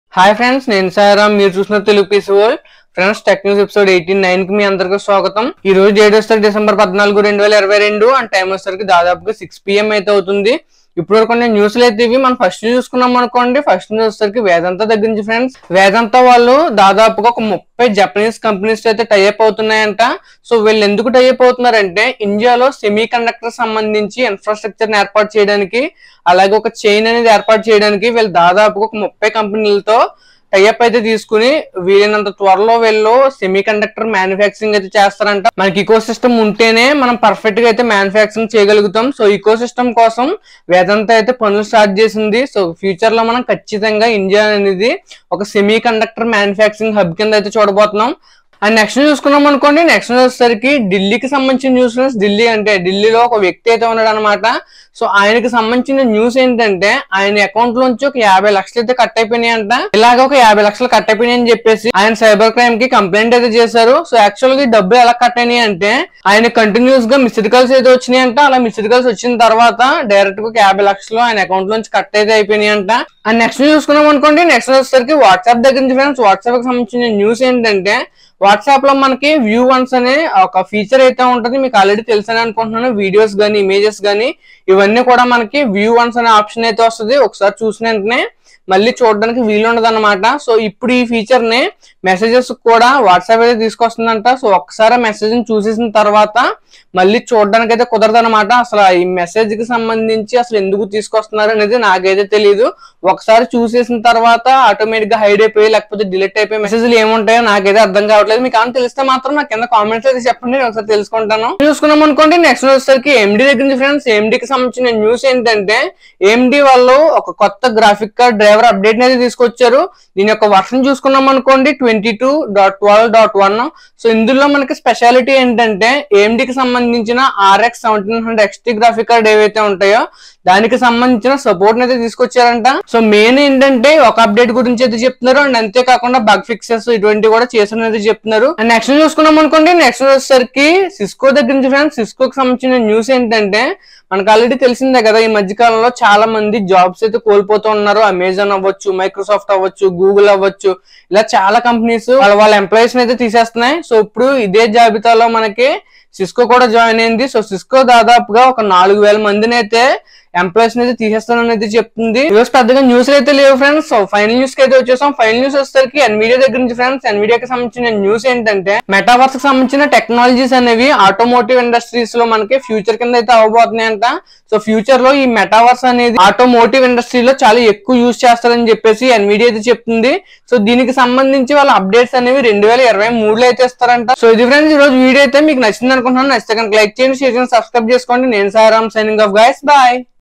Tech News 89 | vedanta semicondutor news, New scam,intel and amd news
HI friends TELUGU PC WORLD ki swagatham.